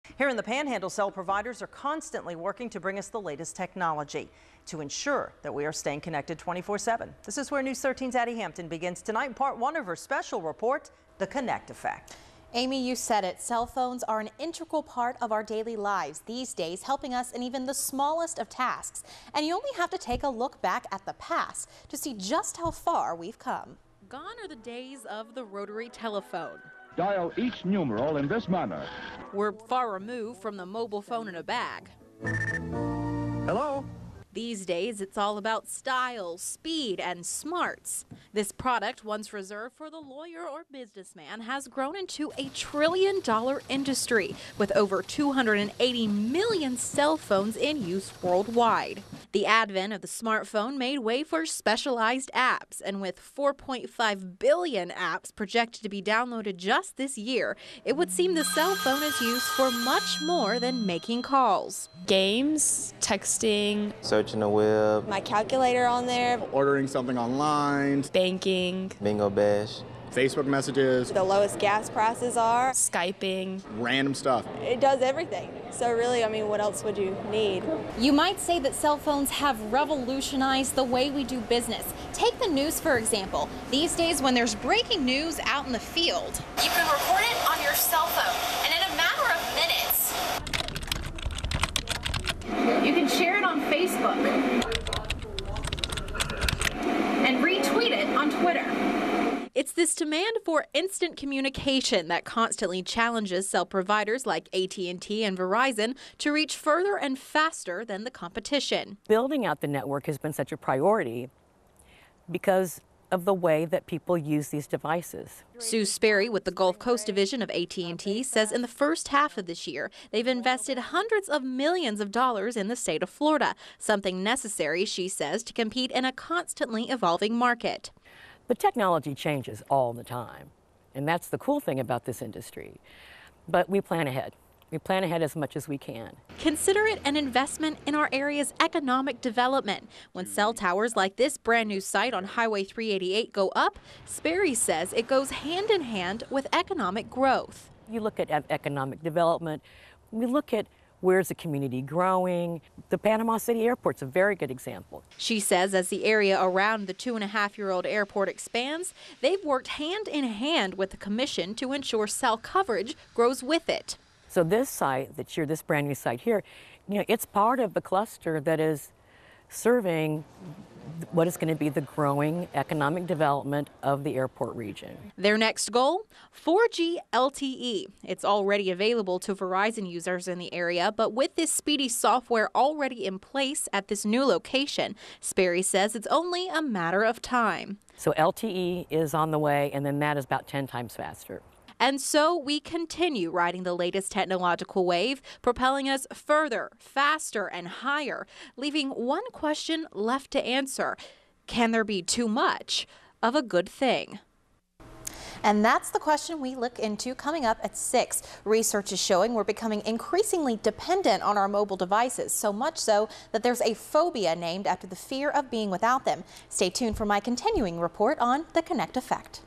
Addie Hampton, WMBB Special Report: "The Connect Effect: Revolution"
We use them everyday: our cell phones. Some might say they've revolutionized our daily lives; from the office, to school and everywhere in between.

It's our move towards an ever-growing mobile society that inspired News 13 to look into the influence cell phones have had on the way we communicate.

Here in the Florida Panhandle, cell providers are constantly working to bring us the latest technological advances; ensuring that we are staying connected 24-7.

These days, cell phones help us in even the smallest of tasks. You only have to take a look at the past, to see just how far we've come.

Gone are the days of the rotary telephone, and we're far removed from the mobile phone in a bag.

This product once reserved for the lawyer or businessman has grown into a trillion dollar industry, with over 280 million cell phones in use worldwide.

These days it's all about style, speed and smarts.

The advent of the smart phone made way for specialized apps, and with 4.5 billion apps projected to be downloaded this year, it would seem the cell phone is used for much more than making calls.

You might say cell phones have revolutionized the way we do business. Take the news business, for example. These days when there's breaking news out in the field, you can record it on your cell phone and in a matter of minutes, you can share it on Facebook and re-tweet it on Twitter.

It's this demand for instant communication that constantly challenges cell providers like AT&T and Verizon to reach further and faster than the competition.

"Building up the network has been such a priority, because of the way people use these devices," said Sue Sperry with the Gulf Coast Division of ATT.

She said in the first half of this year, they've invested hundreds of millions in Florida. It is something necessary, she said, to compete in a constantly evolving market.

"The technology changes all the time and that's the cool thing about this industry, but we plan ahead. We plan ahead as much as we can," said Sperry.

Consider it an investment in our area's economic development. When cell towers like the brand new cite on highway 388 go up, Sperry said it goes hand in hand with economic growth.

"You look at economic development. You look at where the community is growing. The Panama City Airport's a very good example," she added.

Sperry said as the area around the 2.5 year old airport expands, they've worked hand in hand with the commission to ensure cell coverage grows with it.

"This brand new site is part of the cluster that is serving what is going to be the growing economic development of the airport region," said Sperry.

Their next goal is 4g LTE.

It's already available to Verizon users, but with the speedy software already in place at this new location, Sperry said it's a matter of time until it's available to AT&T users.

"[LTE] is about 10 times faster," said Sperry.

So, we continue riding the latest technological wave that propels us further, faster, and higher.

It leaves one question left to answer: can there be too much of a good thing?

Research is showing we're becoming increasingly dependent on our mobile devices. So much so, that there's a phobia named after the fear of being with out them.

For more, go to Part 2 of this News 13 Special Report, "The Connect Effect: Obsession"